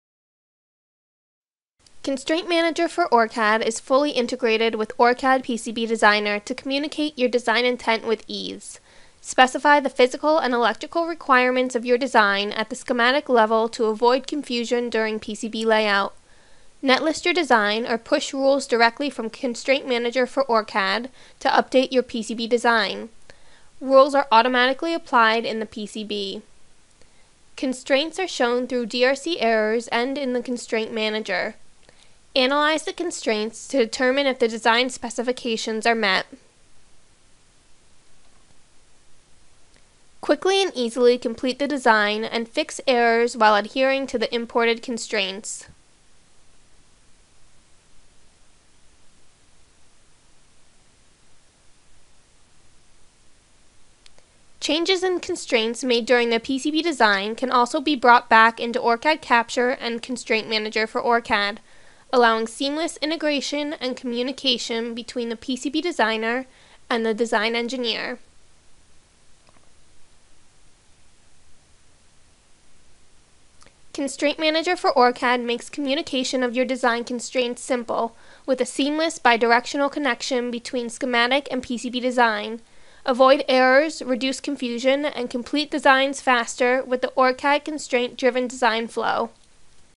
Constraint Manager for OrCAD Overview: Part 3
This video provides an overview of Constraint Manager for OrCAD and its integration with OrCAD PCB Editor.

Constraint Manager for OrCAD makes your design constraints an inherent part of your process. Easily communicate design intent throughout design development.